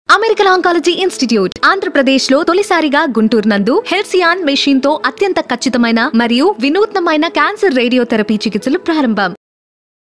Varian Halcyon, World's Most Advanced Radiation Therapy System | American Oncology Institute, Guntur
In a bid to provide exemplary cancer treatment, American Oncology Institute recently installed the world's most advanced radiation therapy system in its Guntur centre. This is the first hospital in Andhra Pradesh to offer the latest technology and facilities in cancer treatment, backed by medical expertise of international repute. The installation of Halcyon is a major milestone for American Oncology Institute, lending more weight to their vision of providing precision cancer care. With Halcyon, AOI is poised to help millions in their fight against cancer.